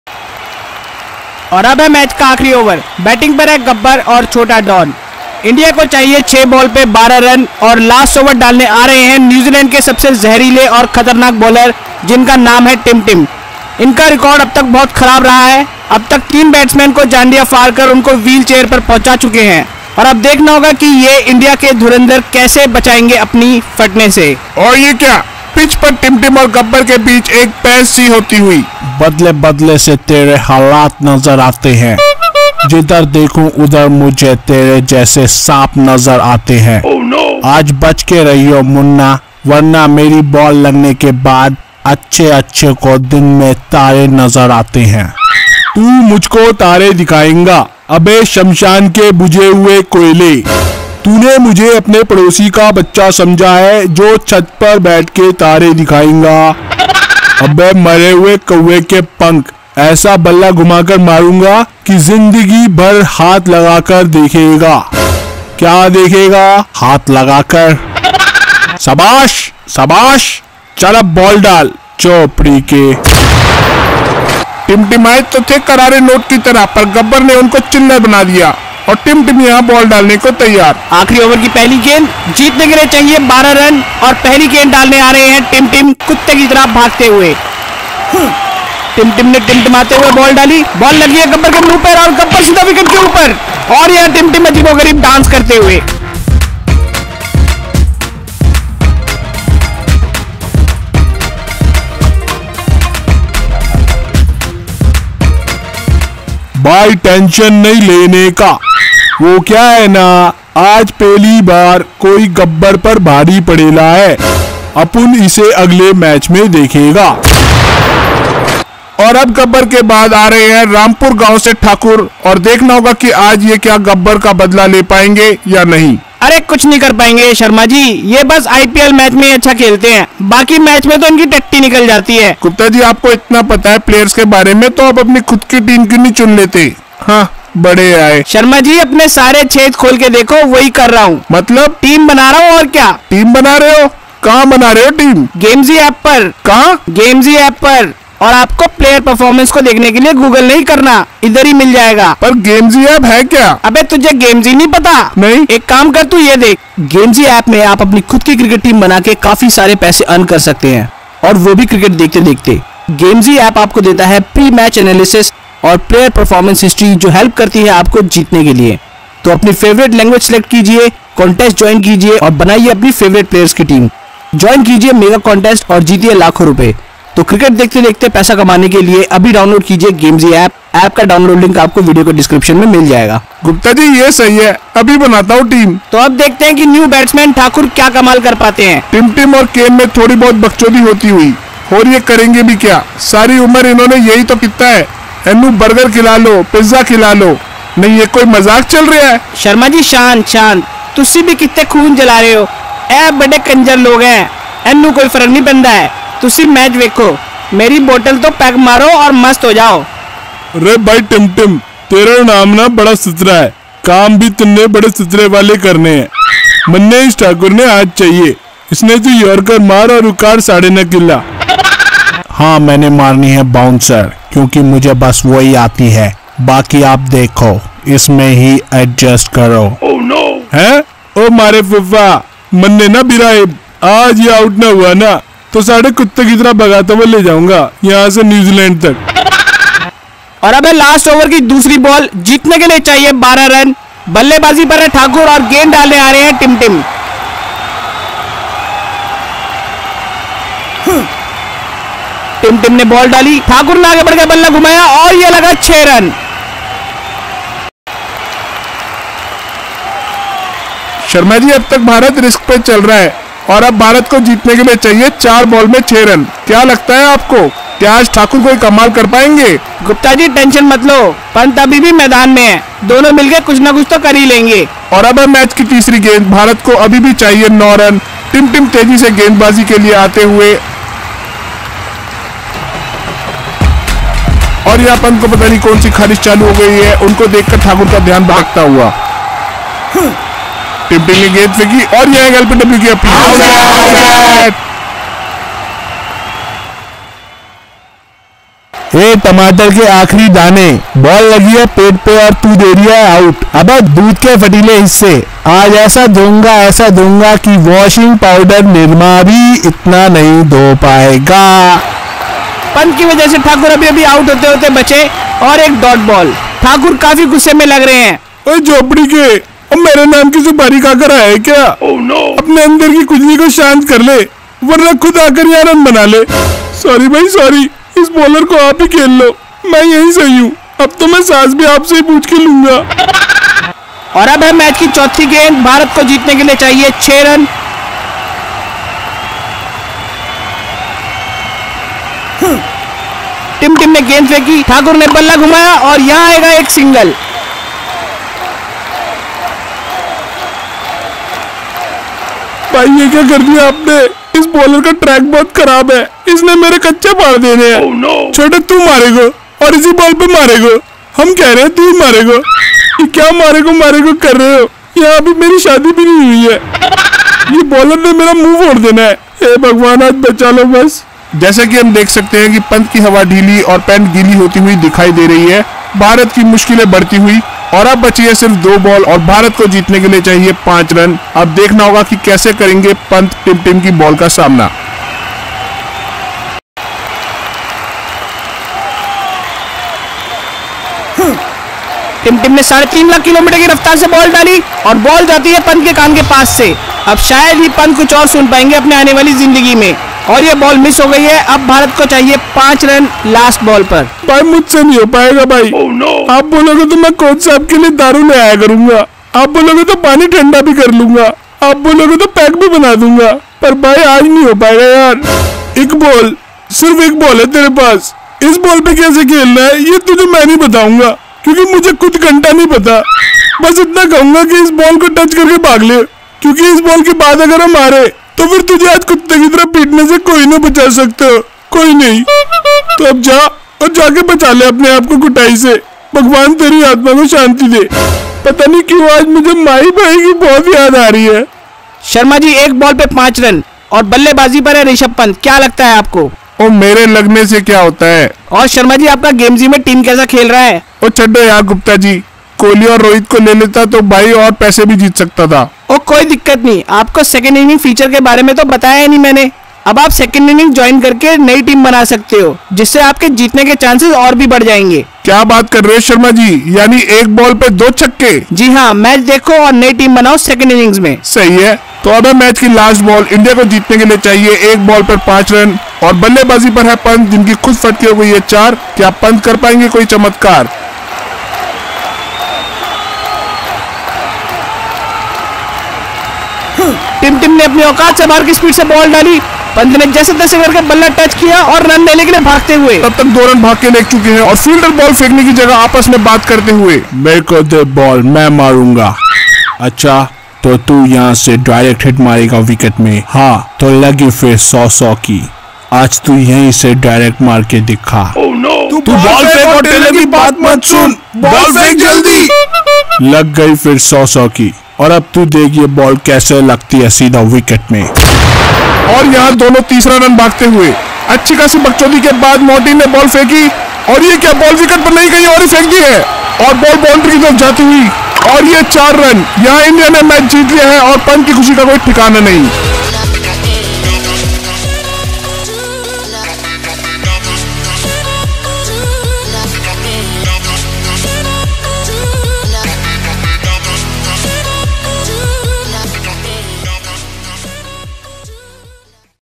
3D ANIM COMEDY - CRICKET INDIA VS NEWZEALAND || LAST OVER || ODI
Watch CRICKET INDIA VS NEWZEALAND LAST OVER ODI
Rs. 50 signup bonus. Play Cricket matches & win cash.

Next Video will be Available soon.. Keep Watching.. Keep Smiling..

BG Music & Sound Effects - SoundEffectsFactory

|| Follow Us ||